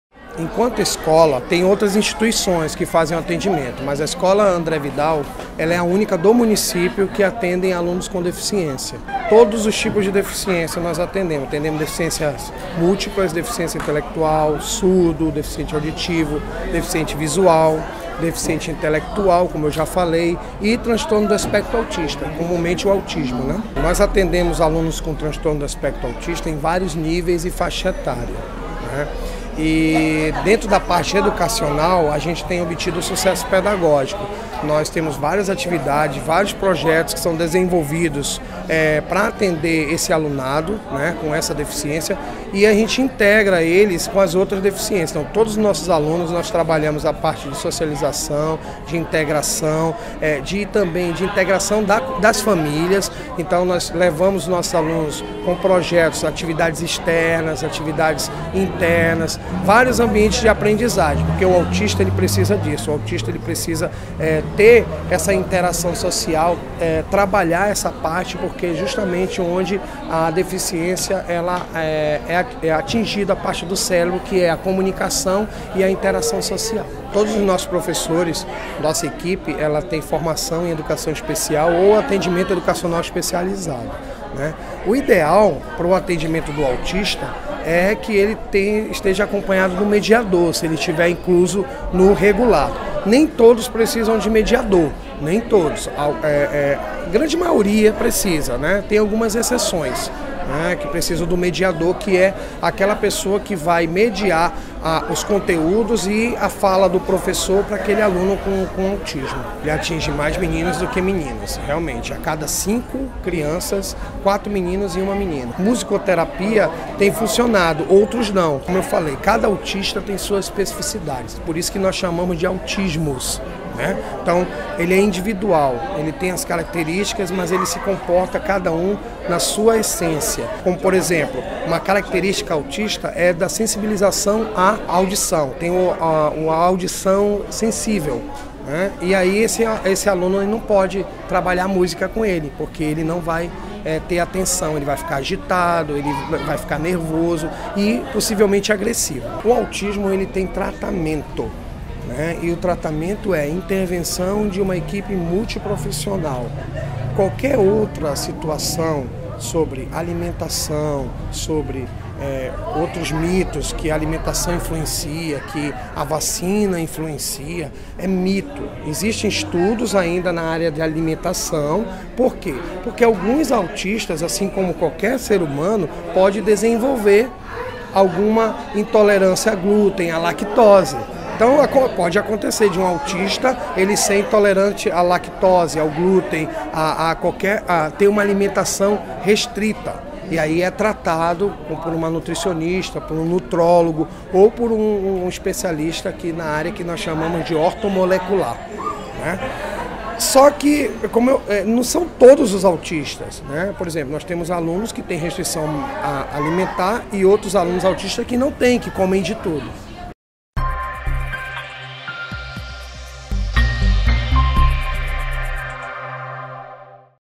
Educador explica como as escolas devem se portar para alunos autistas
No Dia Mundial do Autismo, Helivan Dantas, gestor da única escola que atende crianças especiais em Manaus, diz que é necessário o investimento de um acompanhamento especializado e uma estrutura própria.